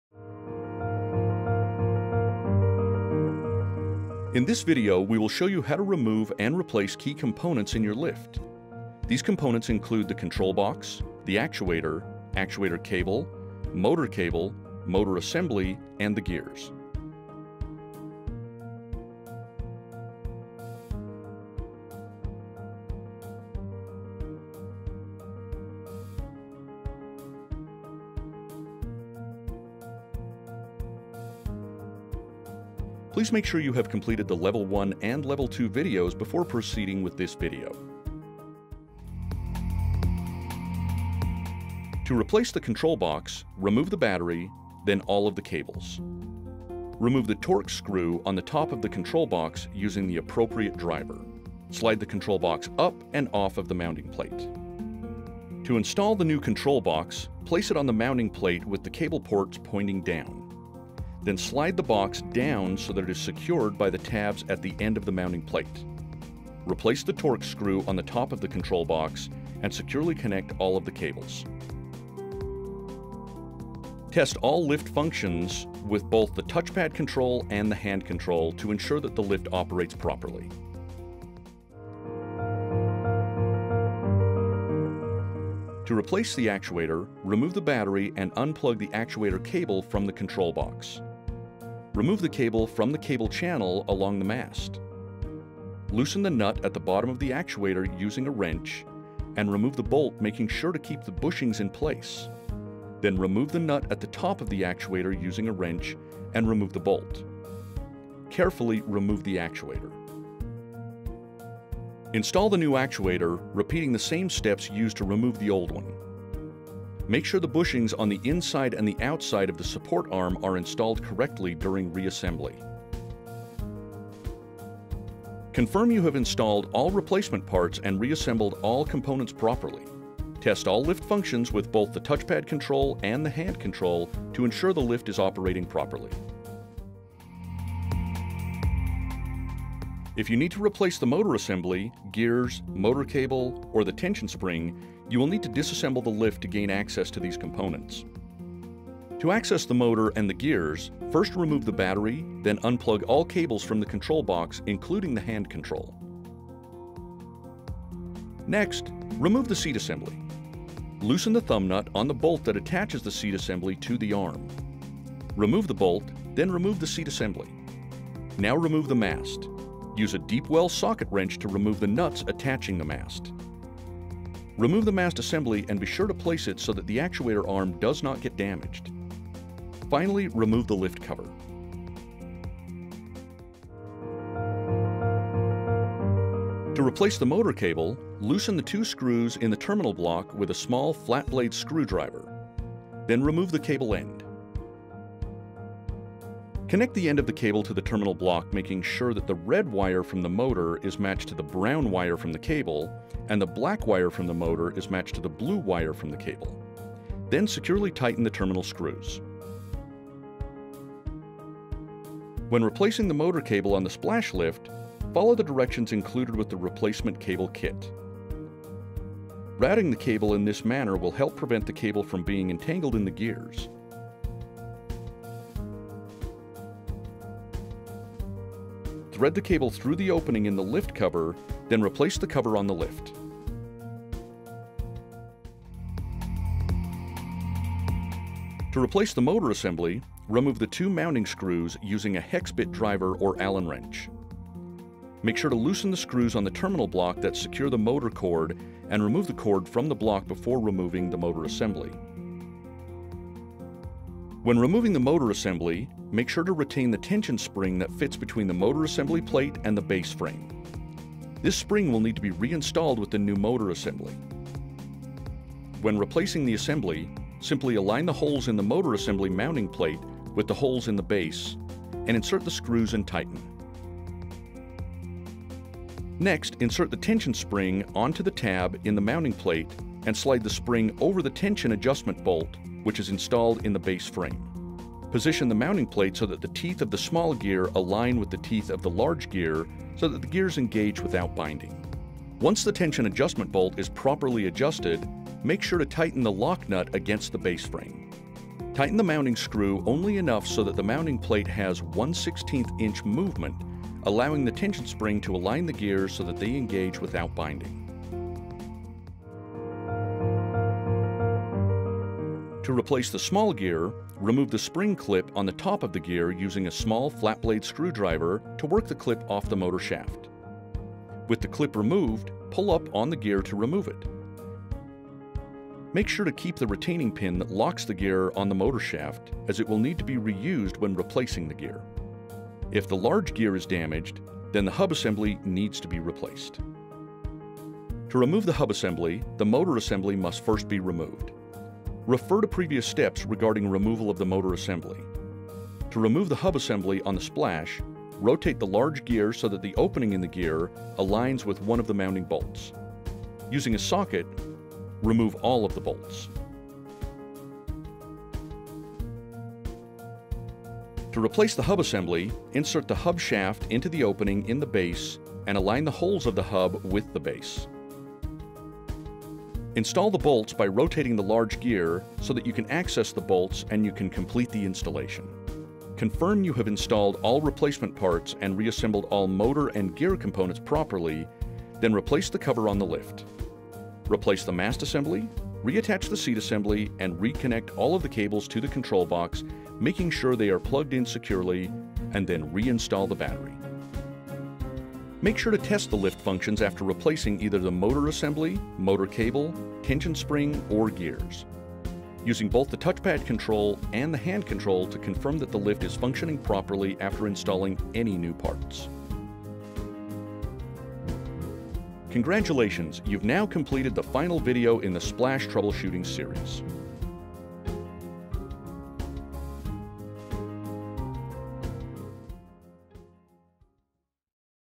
Splash! Pool Lift, Level 3: Resolution
After watching Splash! Pool Lift Troubleshooting Levels 1 & 2, proceed with this video to learn how to  remove and replace key components of your Splash! pool lift such as the control box, actuator, actuator cable, motor cable, motor assembly, and gear.

For more information on the Splash! Pool Lift